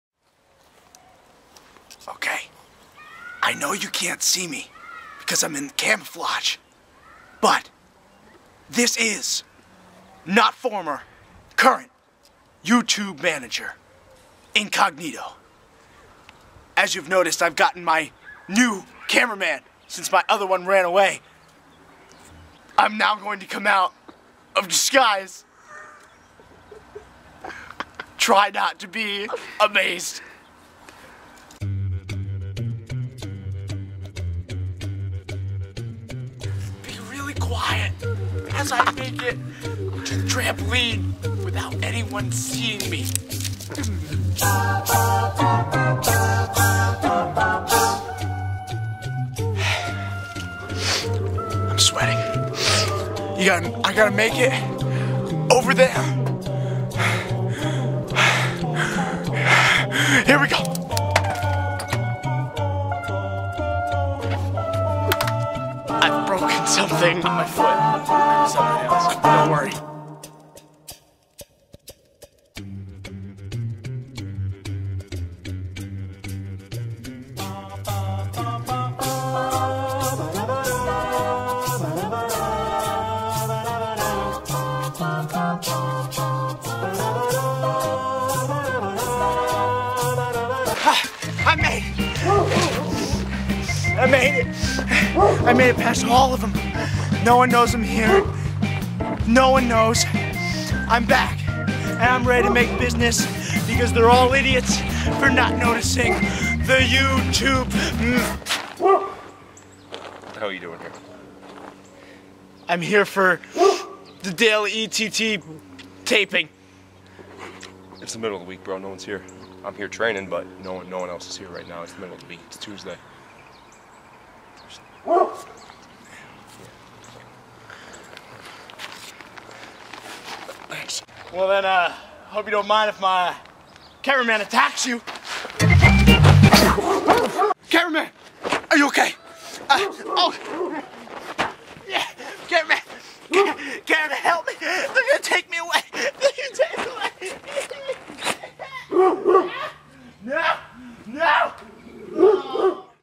ETTW | The (Former) YouTube Manager trying to sneak into ETTW
After being kicked out of ETTW for good, the former YouTube manager puts on a tsssguise and tries to sneak into the "ETT" to reclaim his position as the YouTube manager!